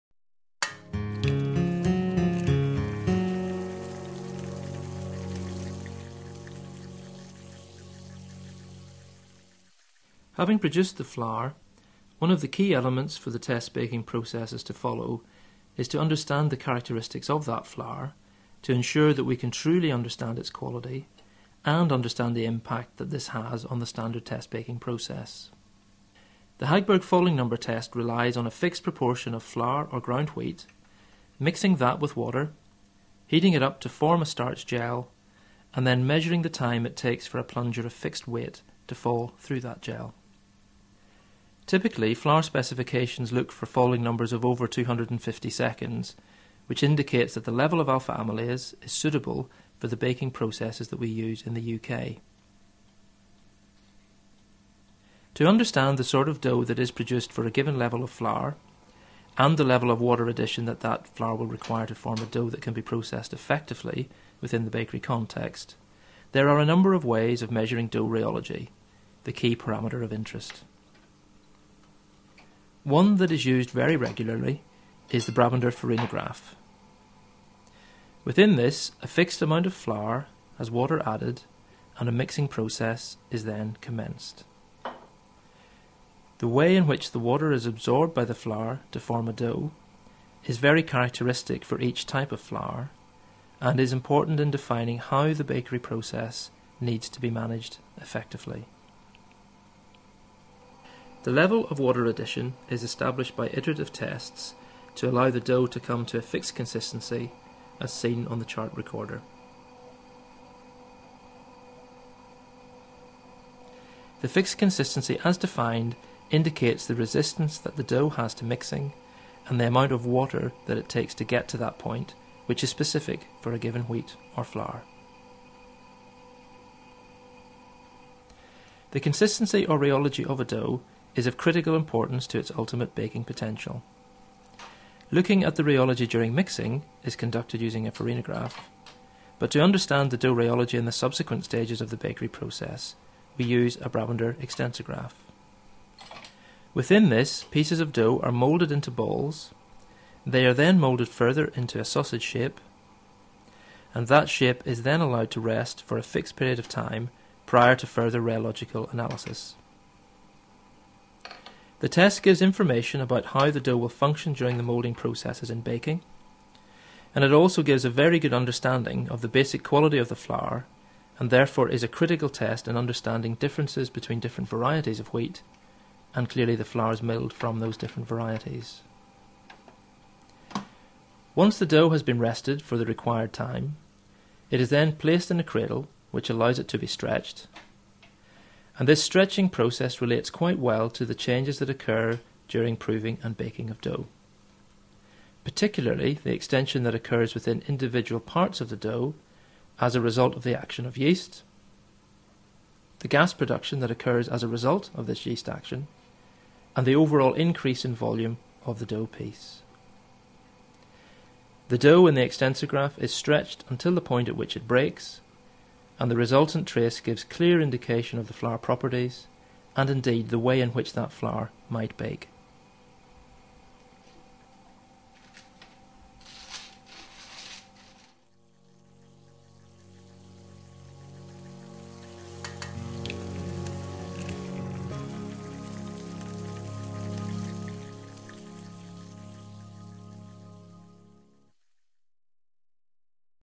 Flour testing | Campden BRI
Flour testing -- assessing the quality of the flour and dough through tests such as the Hagberg Falling Number, Brabender farinograph, and the Brabender extensograph.